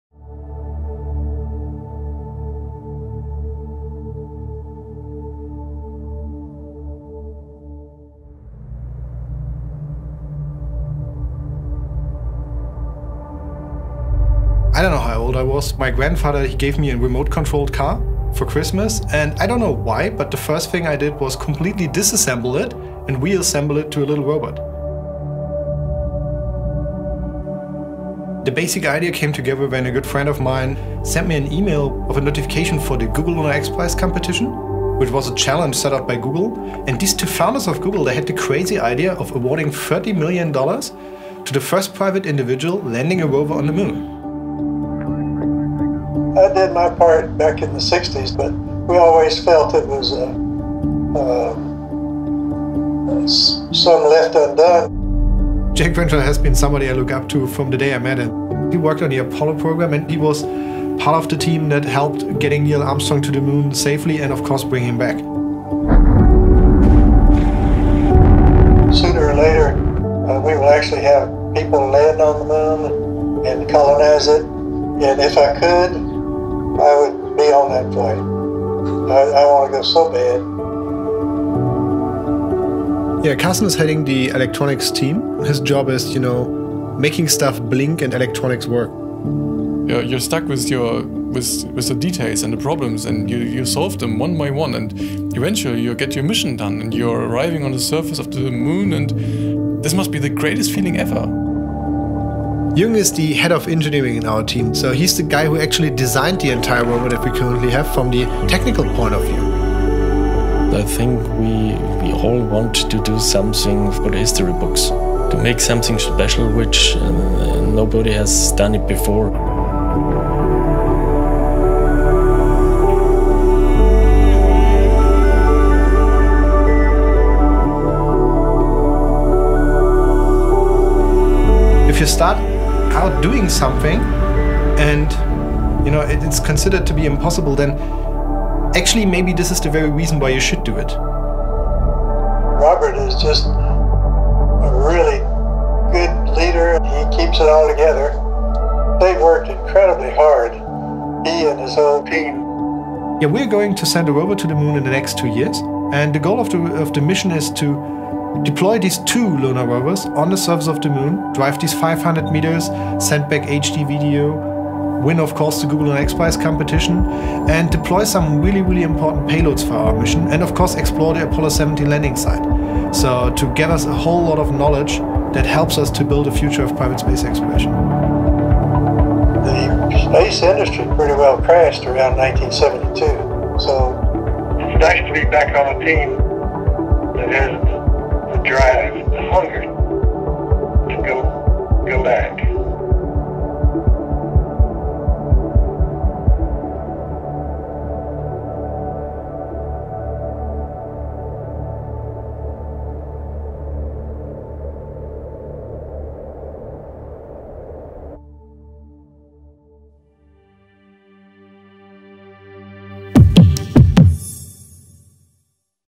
Misión a la Luna: descubre a los Part-Time Scientists
Conoce a Robert Böhme, Karsten Becker, Jürgen Brandner y Jack Crenshaw, protagonistas del equipo que quiere llevar un vehículo lunar Audi a la Luna Subtitulado en español.